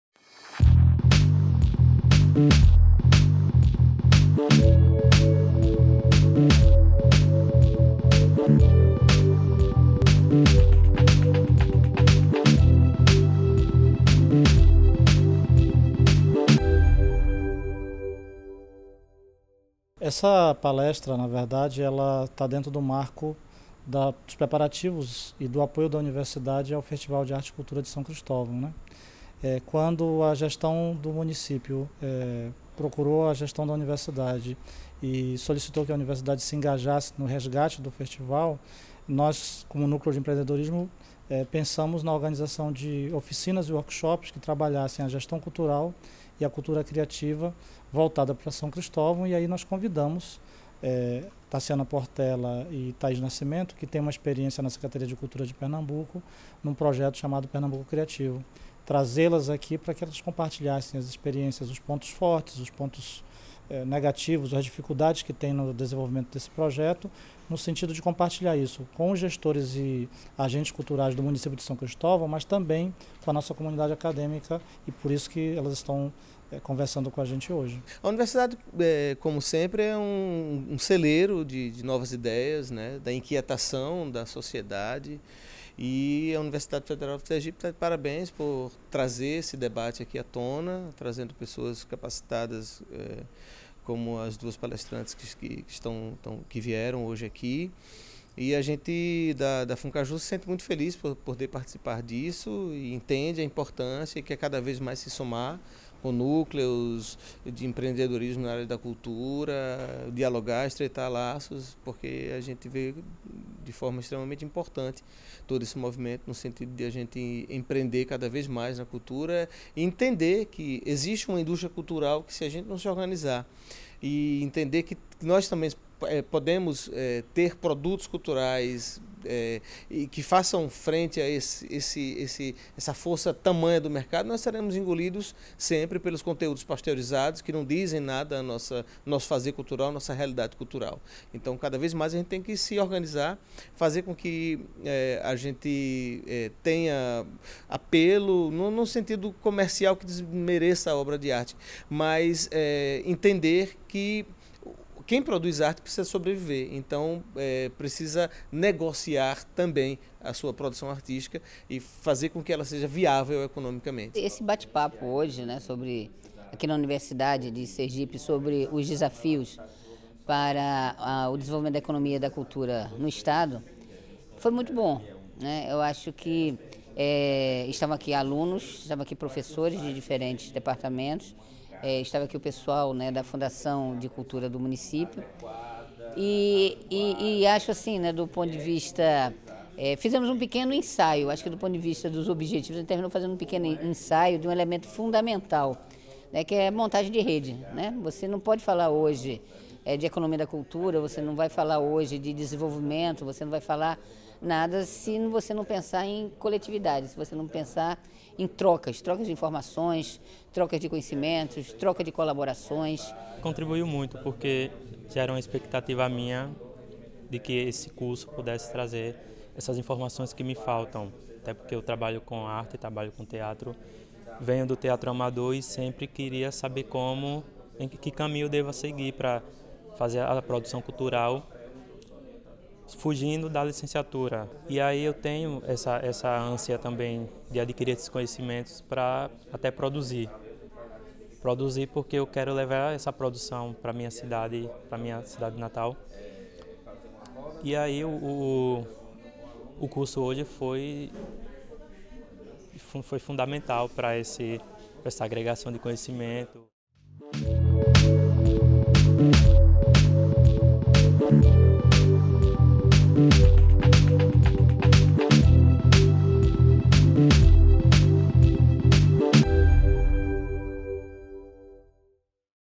Desafios para o Desenvolvimento da Economia da Cultura em Sergipe
Ainda no contexto do retorno do Festival de Arte de São Cristóvão, a Coordenação de Tecnologias Sociais e Ambientais (CTSA), por meio do Núcleo de Empreendedorismo, promoveu a palestra "Desafios para o Desenvolvimento da Economia da Cultura em Sergipe" com Taciana Portella e Thaís Nascimento, do Pernambuco Criativo, onde discutiram alternativas para economia da cultura no estado.

Confira como foi o evento!